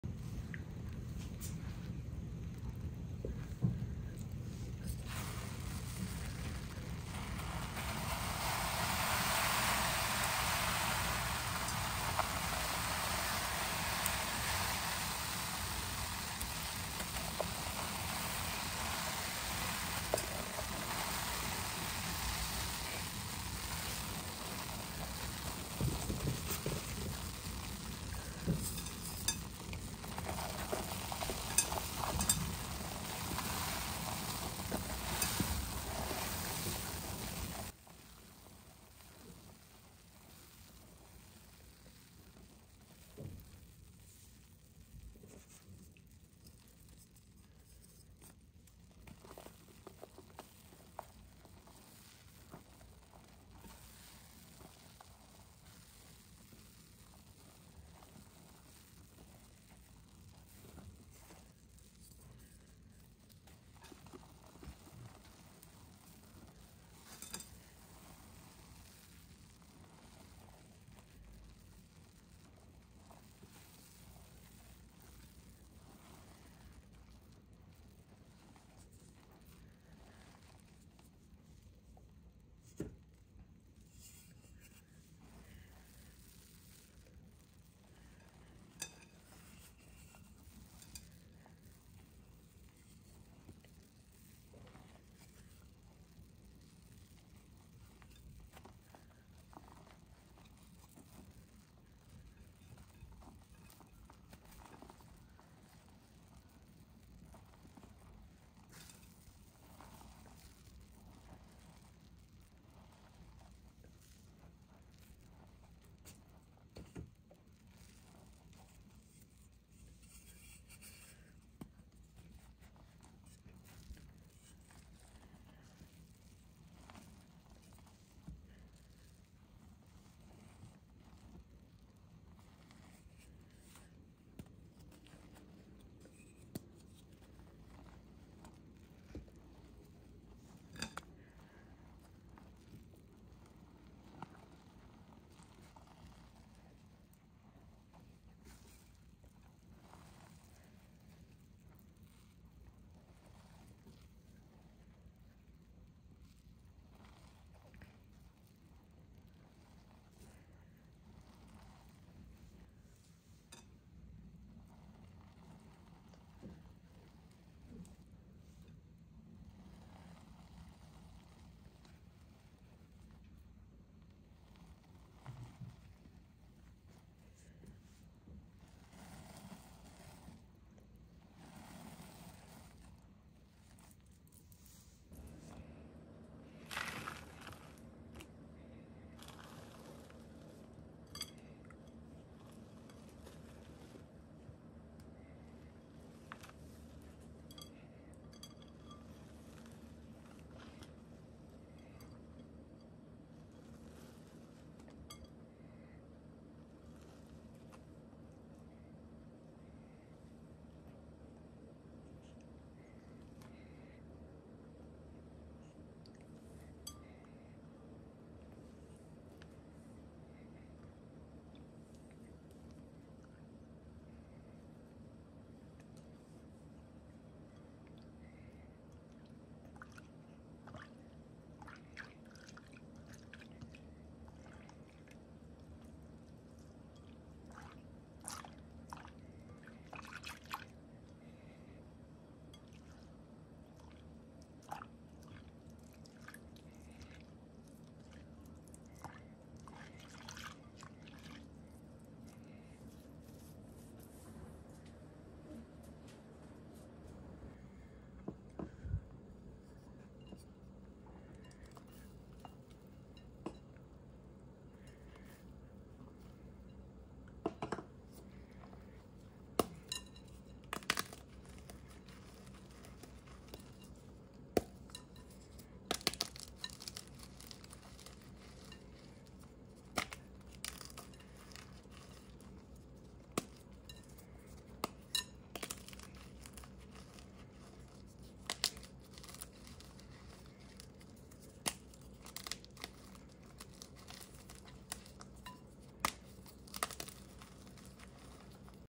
ASMR: HYDROPHOBIC Gym Chalk And Water Crumble
Finallyyyyy got a true hydrophobic mix and I absolutely loved it! This was a fun crush I loved it ☺️☺️